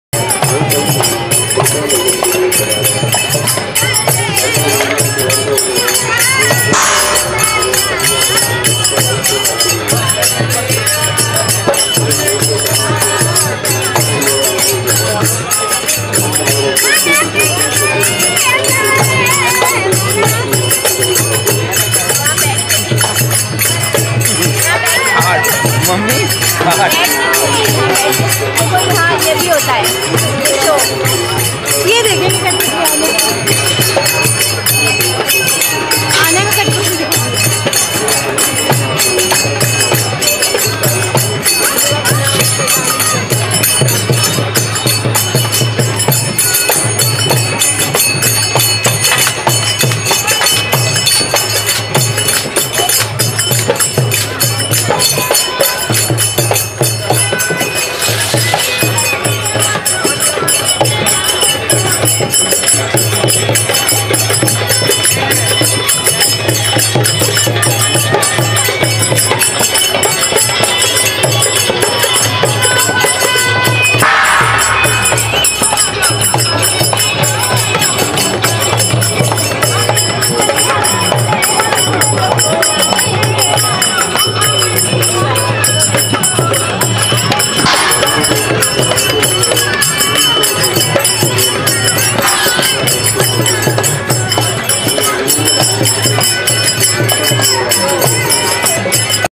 Famous Gagar dance from Rajasthan India
you may see a lot of videos from Rajasthan on my channel because i am really impressed by the rich folk art of Rajasthan. here i am presenting the Gagar dance of Rajasthan which is on e of the famous folk dance from rajasthan.